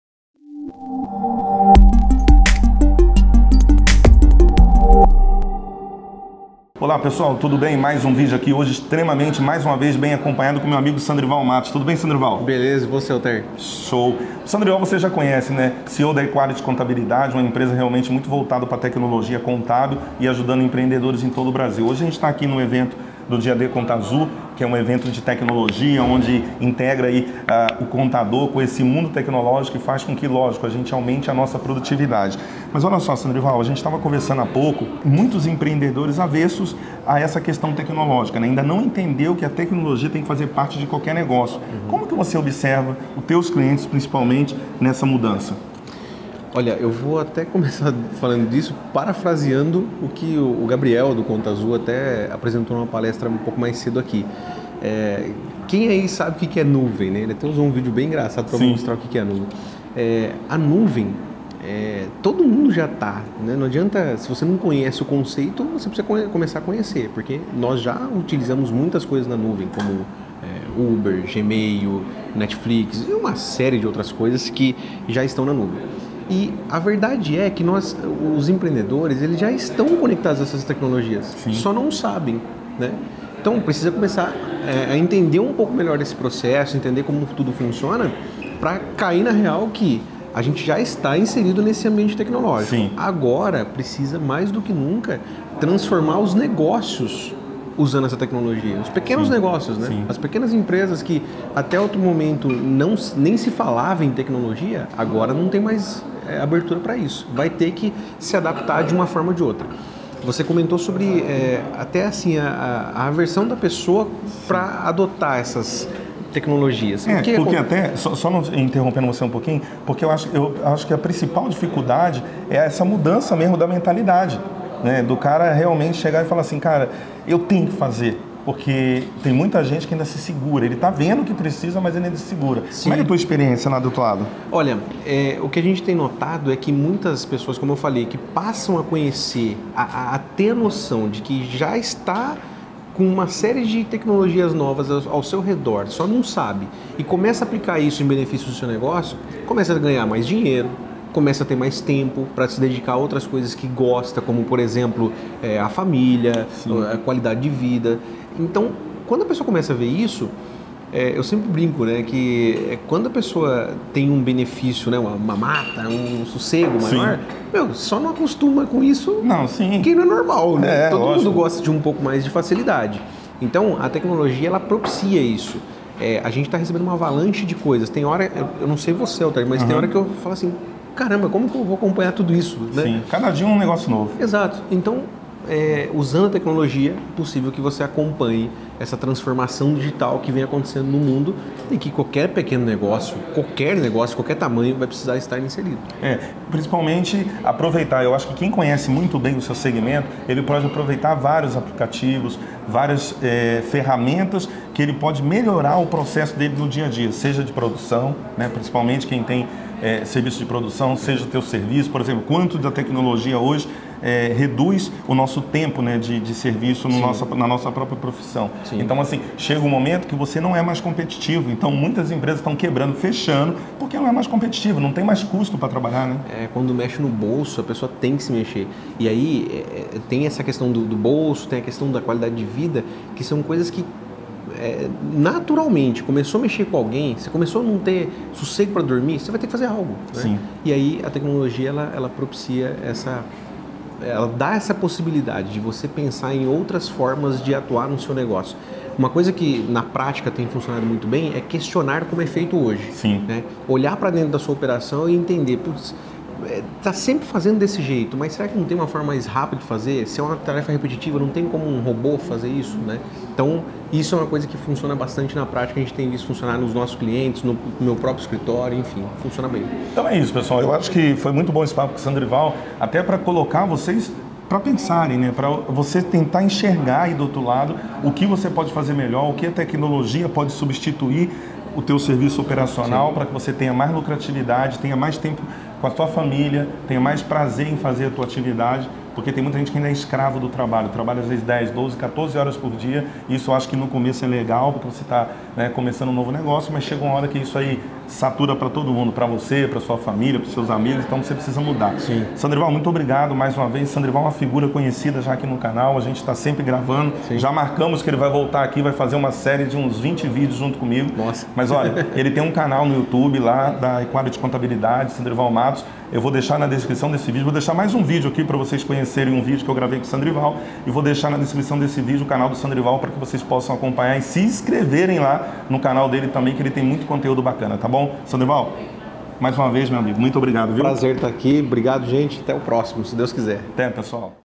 A TECNOLOGIA JÁ FAZ PARTE DA SUA EMPRESA! COM SANDRIVAL MATOS
Olá empreendedores.. Tudo bem?

No vídeo de hoje eu Altair Alves e o CEO da eQuality Contabilidade Sandrival Matos vamos bater um papo em relação a implantação da tecnologia em sua empresa!

Você sabia que a tecnologia já faz parte da sua empresa?
Seja com sistemas em nuvem, arquivos em nuvem, conexões remotas, e etc..